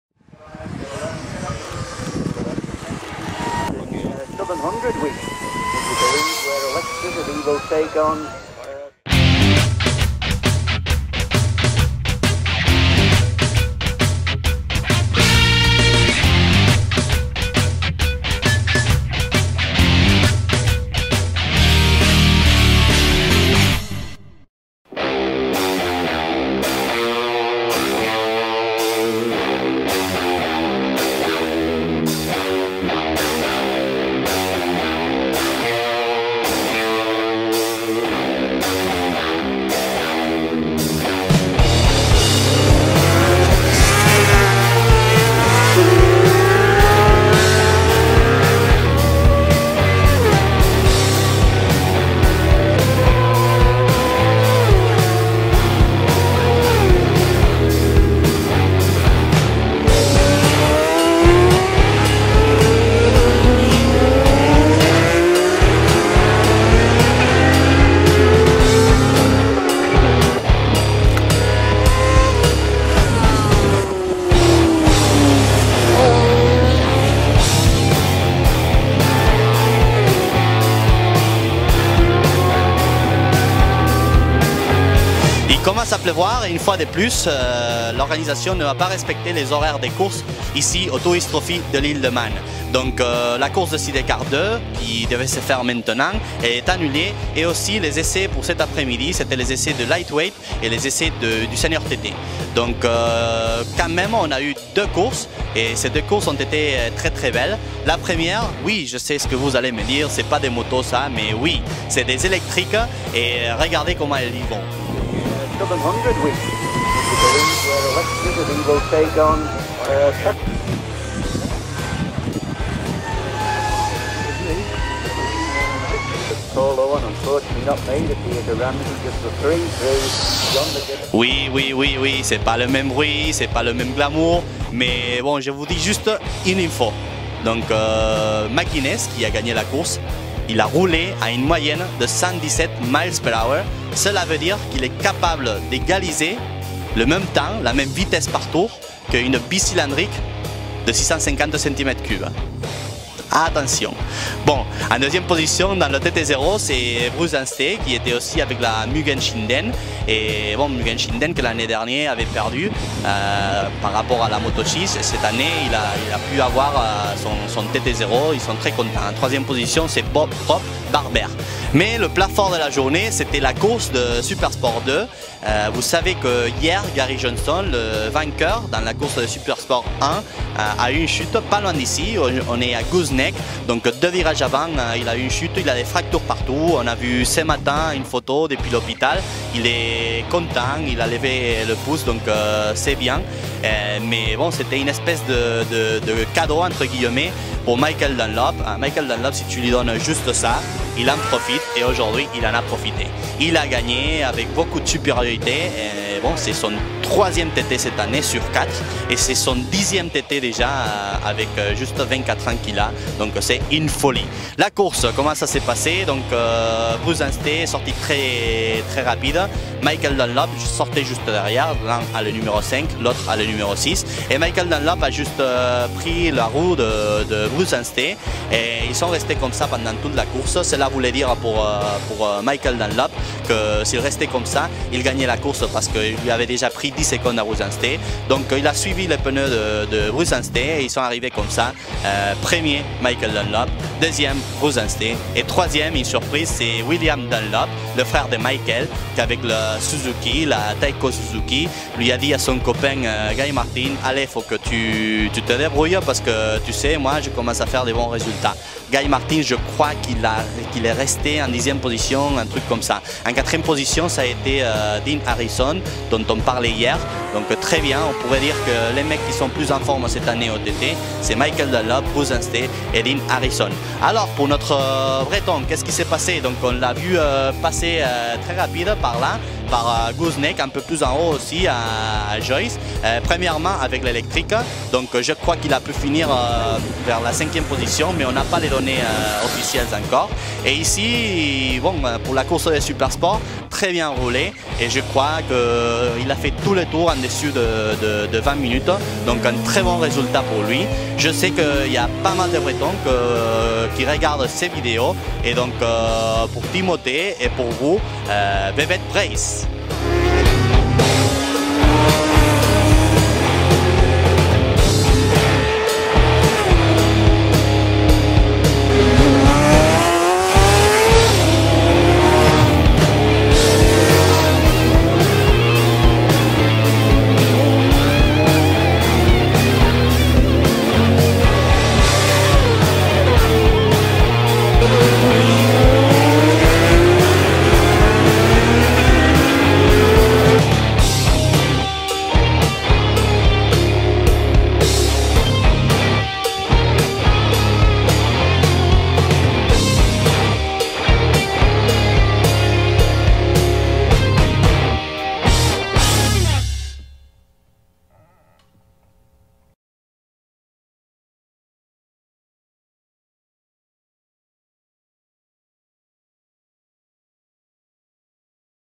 TTMAN14 10 by martimotos.com [FRA]
Deux courses aujourd'hui au Tourist Trophy 2014, le TT Zero avec record incroyable pour une moto électrique, et Supersport 2, avec victoire de Michael Dunlop, le boss du mountain course. Voulez-vous en savoir plus? Ne doûtez pas à regarder cette vidéo!